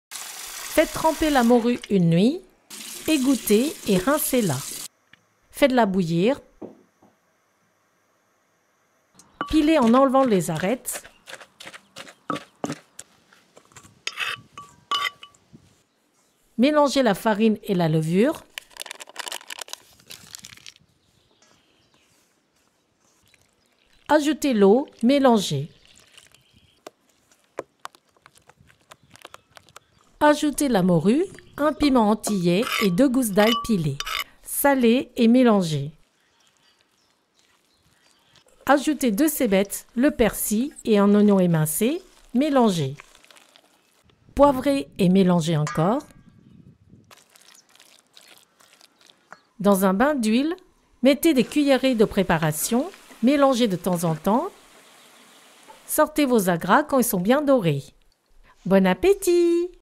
Calendrier de l'Avent J1: 🐟Acras de morue savoureux !
Des acras avec un vrai gout de poisson !
Pour 6 personnes :
500g de morue
20g de persil
2 cébettes ou cives
1 oignon
250g de farine
1 sachet de levure chimique (11g)
400ml d’eau
2 gousses d’ail
1 piment antillais ou 1 mini poivron
½ c à c de sel
¼ c à c de poivre
Huile de friture